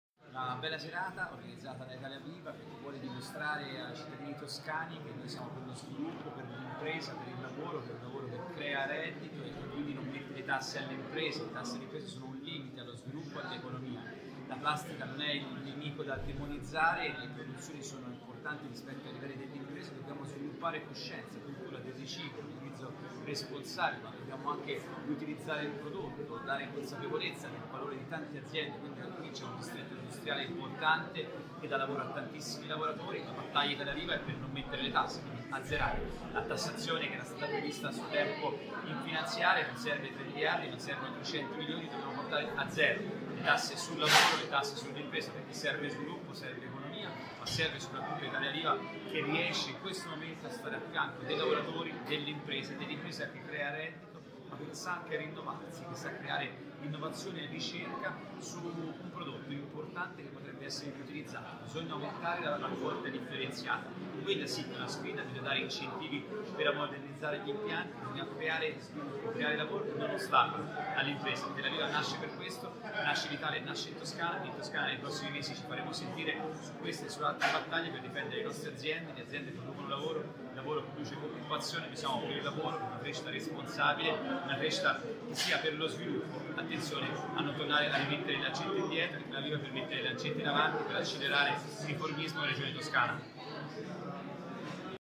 Commento di Stefano Scaramelli consigliere regionale Italia Viva a PRESENTE E FUTURO DELLA PLASTICA
Scaramelli ribadisce la posizione di Italia Viva contro la Plastic Tax che non risolve e rilancia l'importanza dell'innovazione per affrontare i problemi complessi collegati allo sviluppo sostenibile. Aziende, ricerca, ambiente, riciclo sono le parole chiave per l'iniziativa politica di Italia Viva, anche in Toscana dove presto vi saranno elezioni alle quale Italia Viva si presenta autonoma.